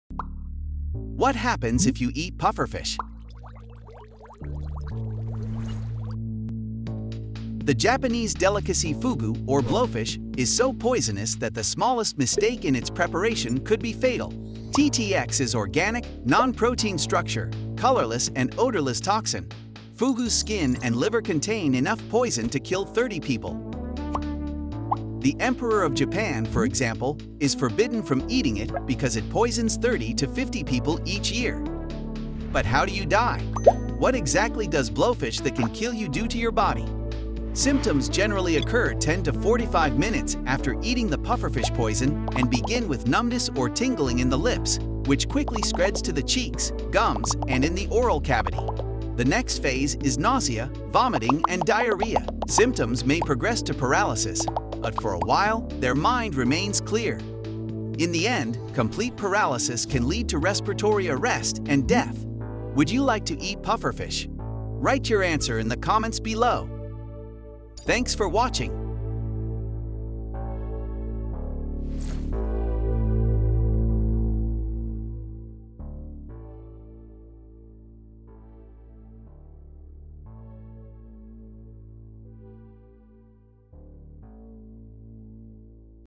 What Happens If You Eat Puffer Fish?
In Japan, the meat of some puffer fish is considered a delicacy- called fugu. It can only be prepared by trained, licensed chefs who know that one bad cut means almost certain death for a customer. In fact, many such deaths occur annually.

So what happens if you eat improperly cooked fish? Watch in our video!

Do you want to see our new videos more often?